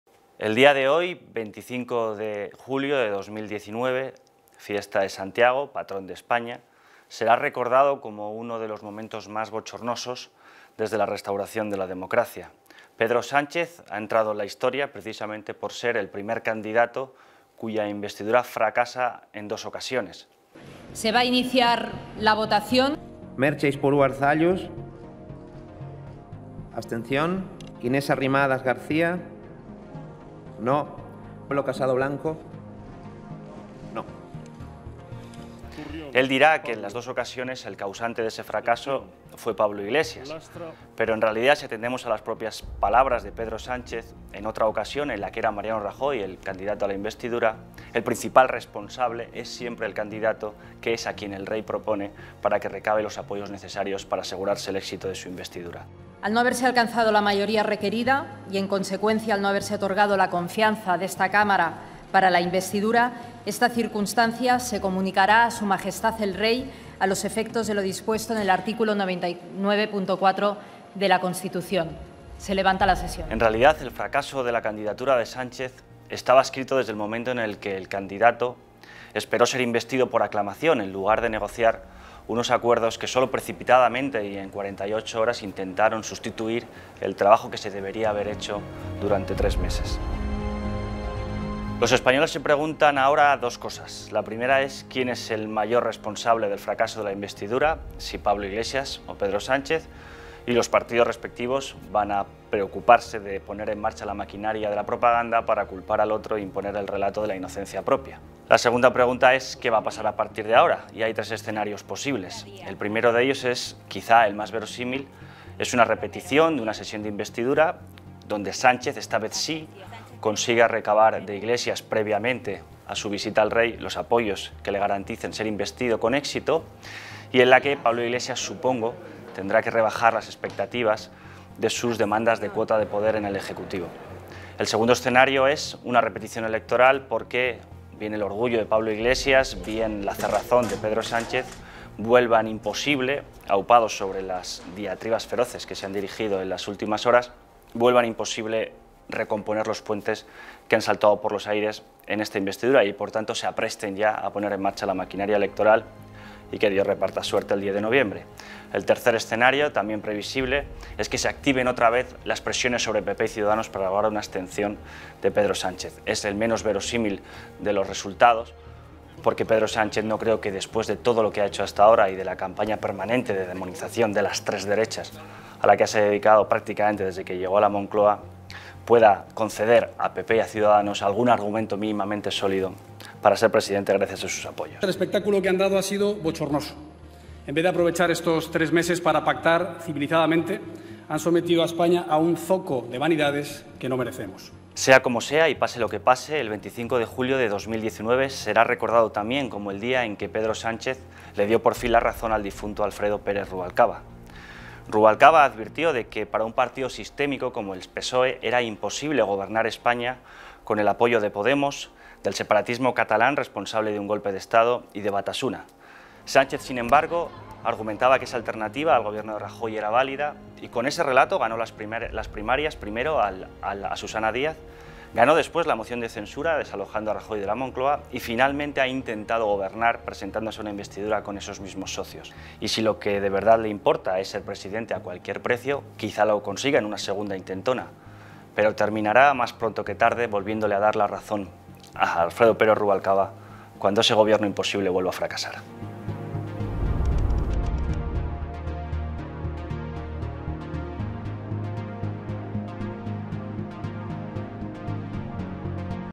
El fracaso de la investidura Frankenstein
Jorge Bustos, jefe de Opinión de El Mundo, analiza la sesión de investidura del 25 de julio, en la que Pedro Sánchez fracasa de nuevo.